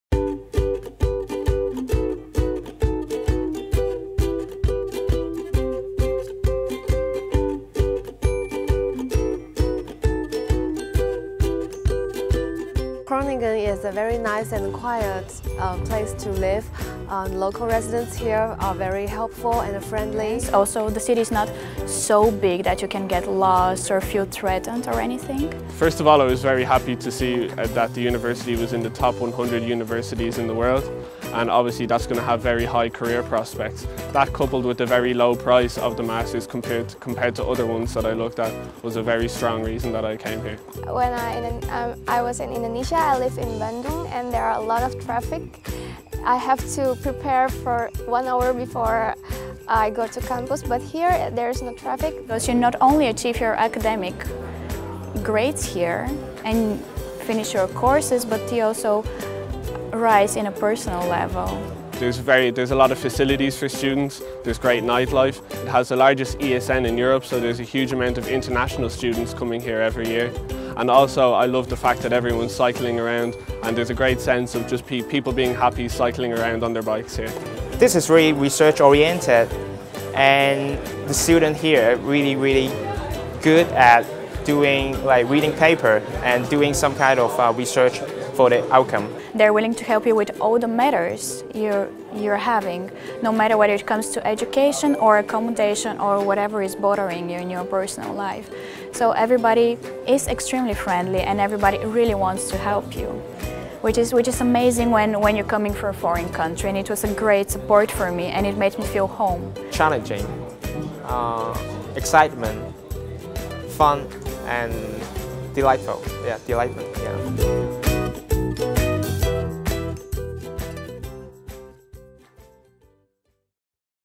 What's great about the Faculty of Economics and Business in Groningen? Listen to our students!
The stories of Annie and Jonathan from China, Eoin from Ireland, Anisha from Indonesia and Hristina from Bulgaria will inspire you to make this life changing decision to study abroad! Why did they choose Groningen? What is it like to live in this friendly student city? Is it easy to get around town? How is the atmosphere at the faculty? How is the level of education?